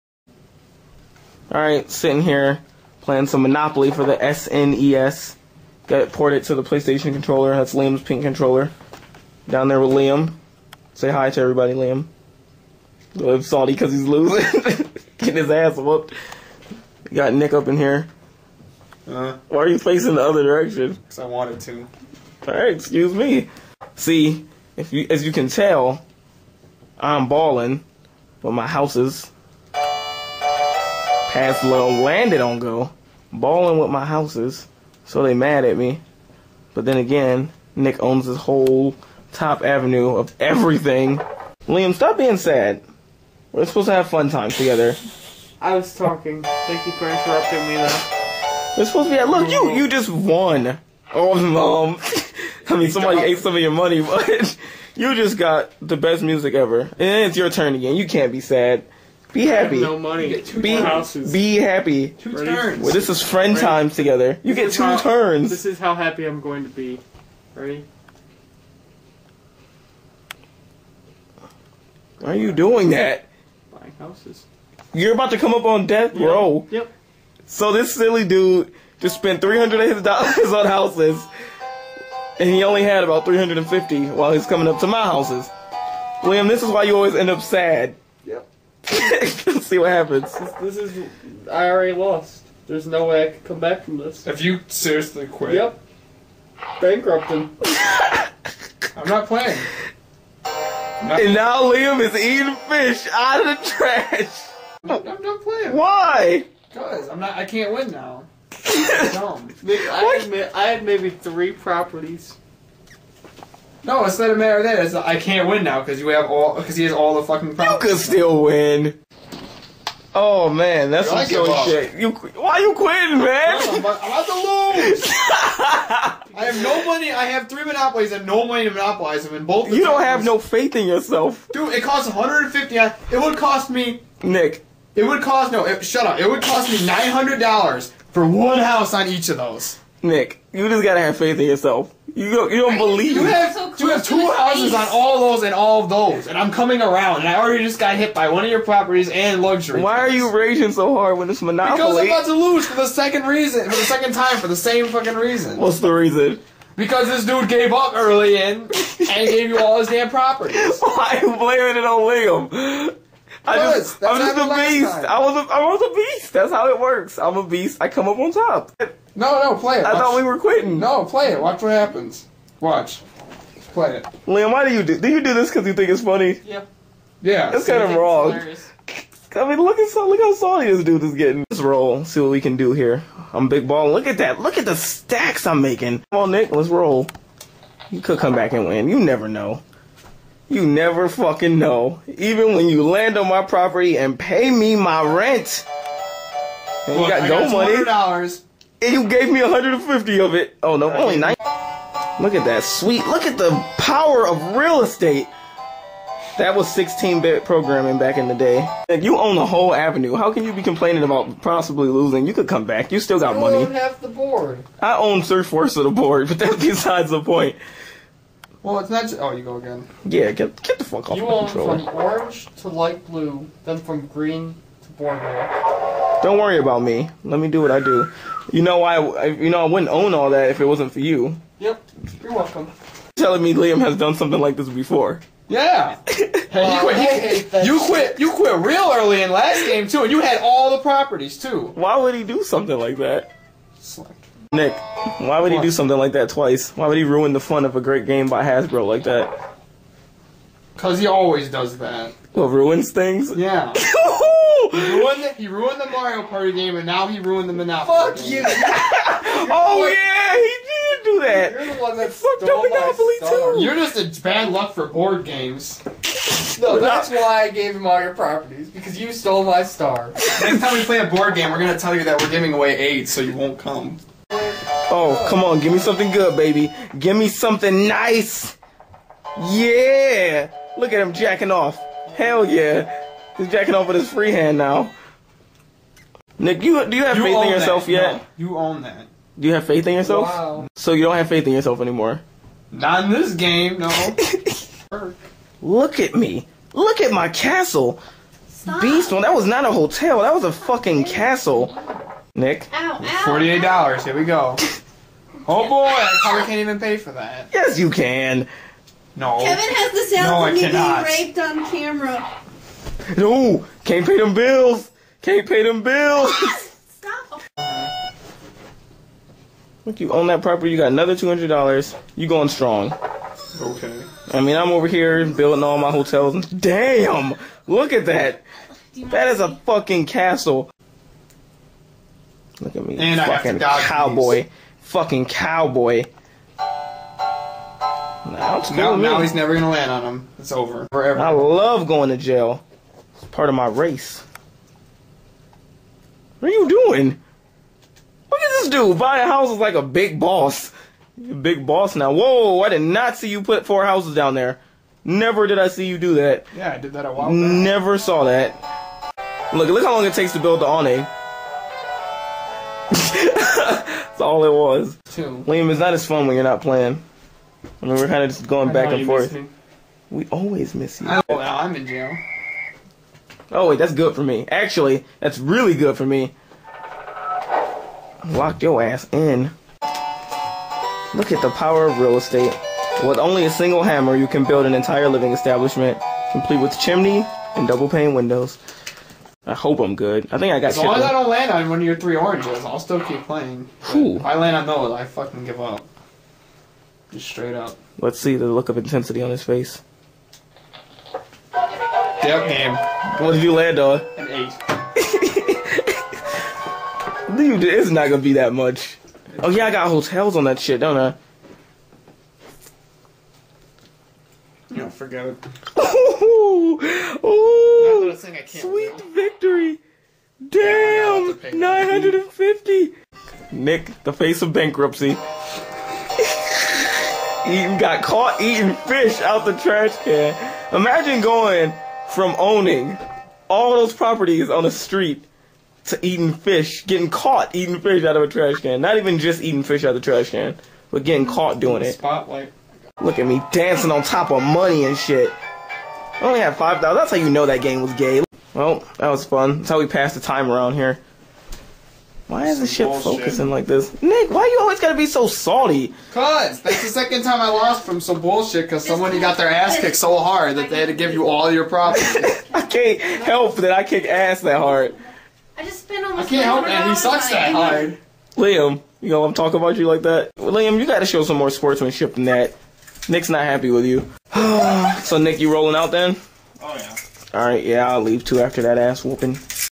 Let's Play Monopoly with Nick, Liam and Kevin
Playin' some good ol' Monops. No, OLP isn't back, just a silly video.